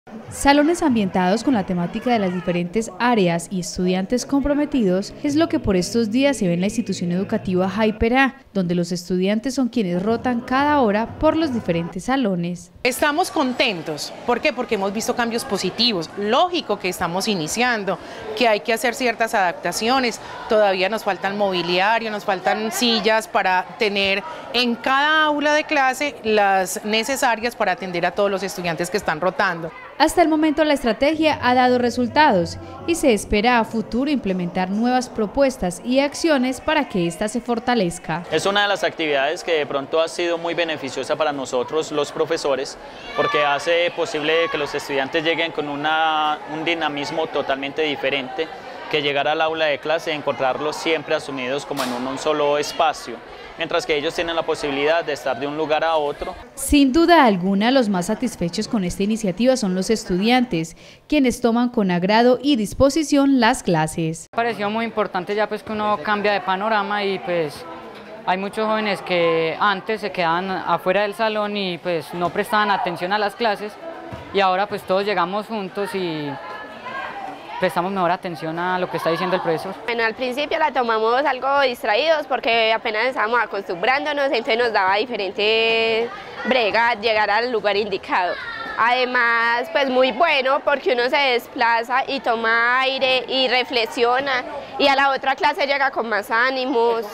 Institución Educativa Jaiperá implementa nueva estrategia para dictar clases
Este año lectivo inicio para la Institución Educativa Jaiperá en Urrao con algunos cambios, que hasta el momento han sido positivos. Ahora los docentes son quienes esperan en el salón de clases a los estudiantes.